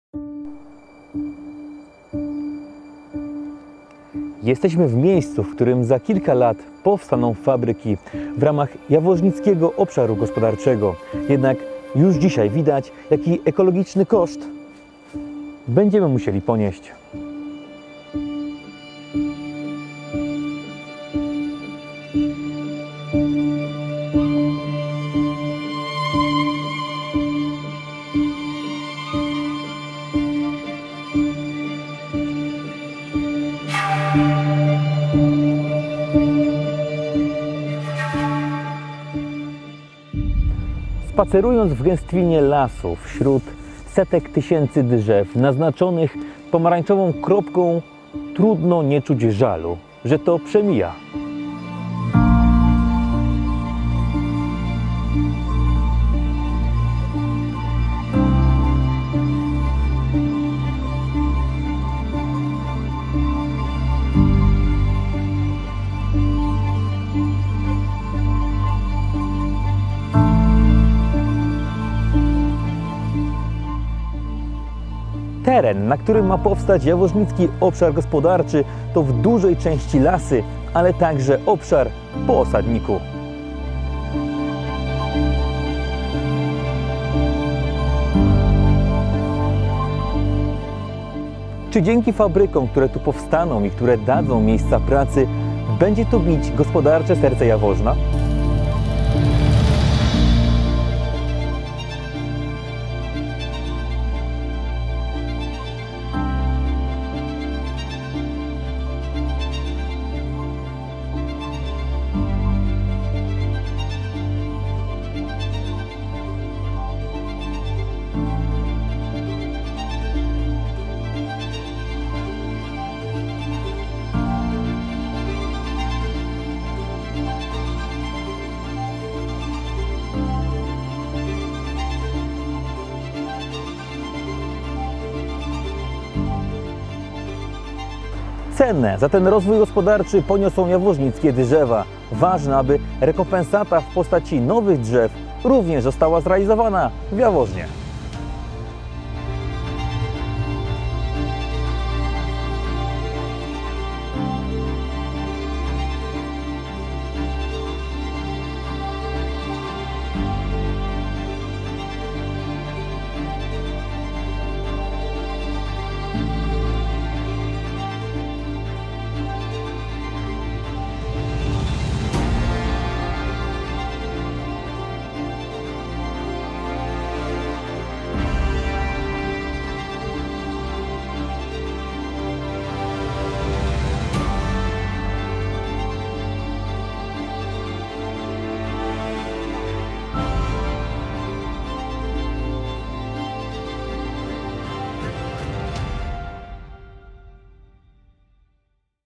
Naznaczone drzewa. Las do wycinki pod fabrykę Izery
Jaworznicki Obszar Gospodarczy, a tym samym fabryka Izery mają powstać na terenie gdzie obecnie jest las oraz terenie gdzie kiedyś mieścił się osadnik polowy. Drzewa do wycinki zostały już naznaczone i jest ich trochę żal, gdy się między nimi spaceruje.

Wybraliśmy się na miejsce, w którym za kilka lat powstaną fabryki w ramach Jaworznickiego Obszaru Gospodarczego i już dzisiaj widać jaki ekologiczny koszt będziemy musieli ponieść, aby zrealizować te inwestycje. Spacerując w gęstwinach lasu, wśród setek tysięcy drzew naznaczonych pomarańczową kropką trudno nie odczuć żalu, że ten zielony obszar przemija.

Teren, na którym ma powstać Jaworznicki Obszar Gospodarczy to głównie teren leśny, ale także obszar po osadniku polowym, czyli miejscu służącym do oczyszczania wody podsadzkowej oraz do gromadzenia wody spływającej z podsadzanych wyrobisk w kopalniach.

Czy dzięki fabrykom, które tu powstaną i które dadzą miejsca pracy, będzie w tym miejscu bić gospodarcze serce Jaworzna?

Największą cenę za ten rozwój gospodarczy poniosą drzewa, ważne jednak, aby rekompensata w postaci nowych drzew, również została zrealizowana w Jaworznie.